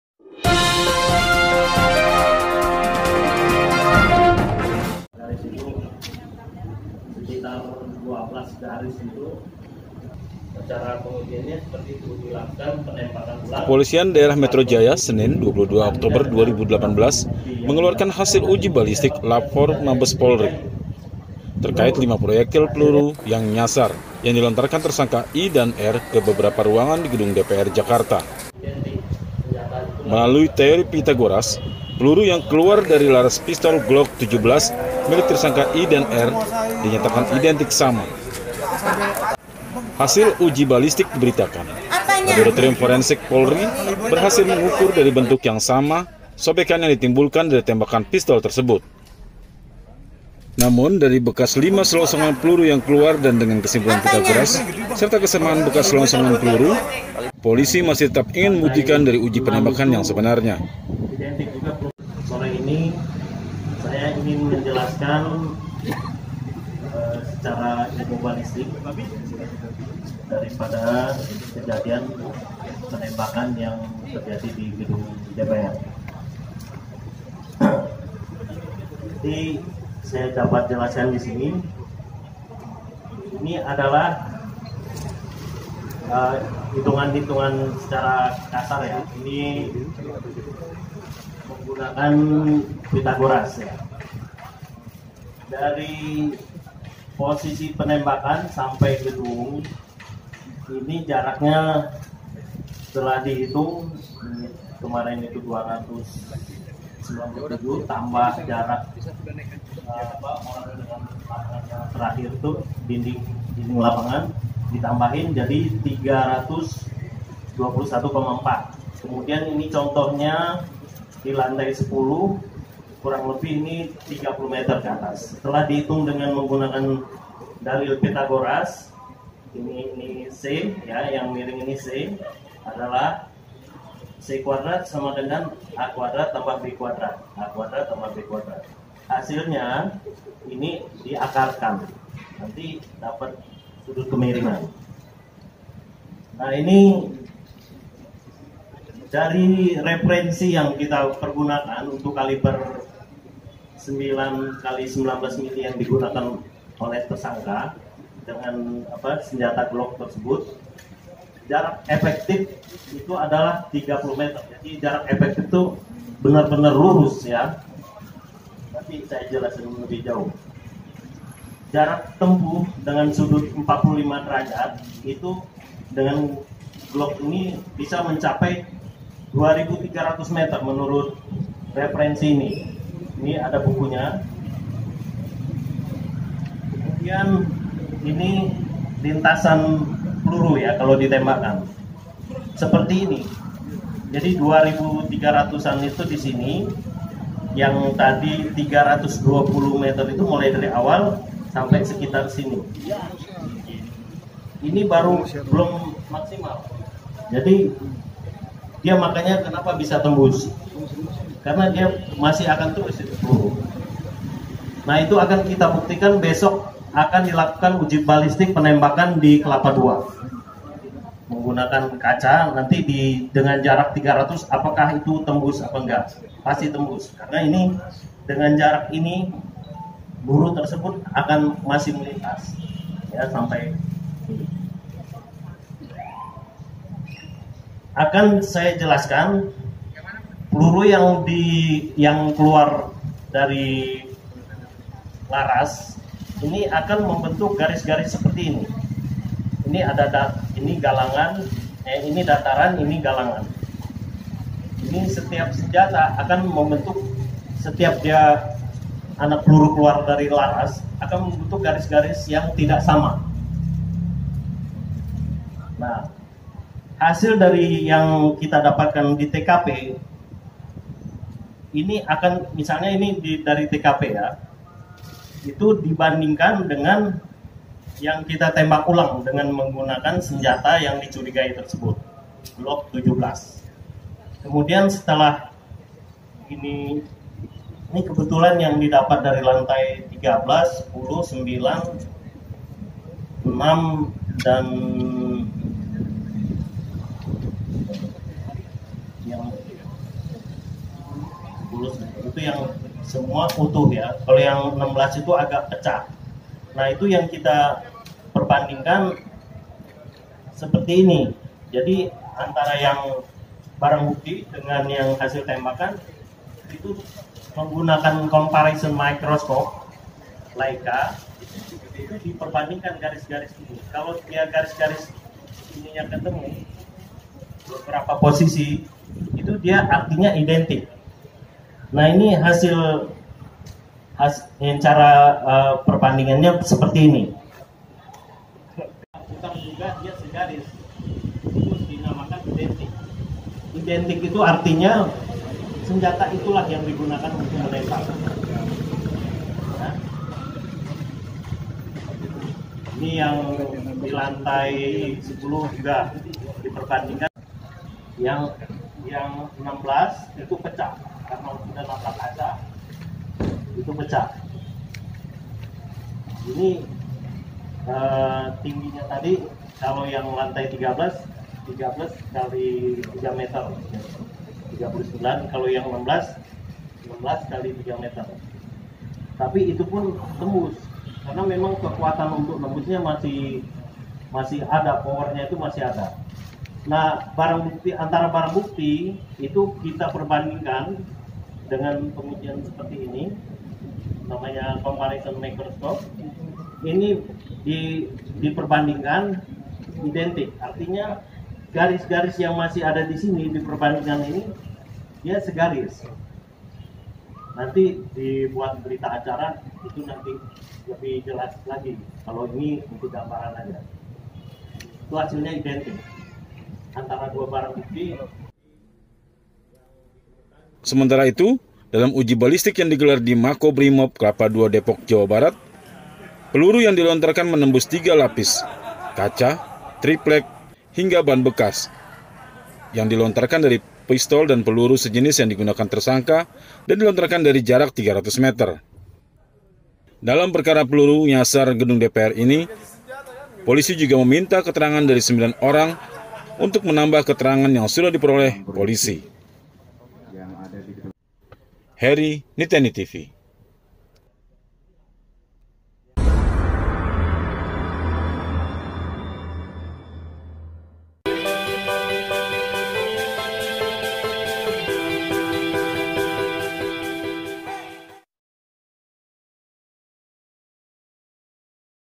Uji balistik Polri pastikan peluru identik dengan tersangka
Kepolisian daerah metro jaya senin, 22 oktober 2018 mengeluarkan hasil uji balistik labfor mabes polri terkait 5 proyektil peluru nyasar yang dilontarkan tersangka I dan R ke beberapa ruangan di gedung dpr jakarta.

Hasil uji balistik diberitakan melalui teori phytagoras peluru yang keluar dari laras pistol glock 17 milik tersAngka i dan r dinyatakan identik sama. Laboratorium forensik polri/ berhasil mengukur dari bentuk sobekan yang ditimbulkan dari tembakan pistol tersebut
dari bekas lima selongsongan peluru yang keluar dan dengan kesimpulan phytagoras  serta kesamaan bekas selongsongan peluru polisi masi tetap ingin membuktikan dari uji penembakan yang sebenarnya.

Sementara itu dalam uji balistik yang di gelar di Mako Brimob kelapa dua depok jawa barat. Peluru yang dilontarkan menembus tiga lapis kaca,  triplek, hingga ban bekas yang dilontarkan dari pistol dan peluru sejenis yang digunakan tersngka dan dilontarkan dari jarak 300 meter.
Sementara itu, dalam perkara peluru nyasar gedung dpr ini polisi juga meminta keterangan dari 9 orang untuk menambah keterangan yang sudah diperoleh polisi.

 Heri/ nitenitv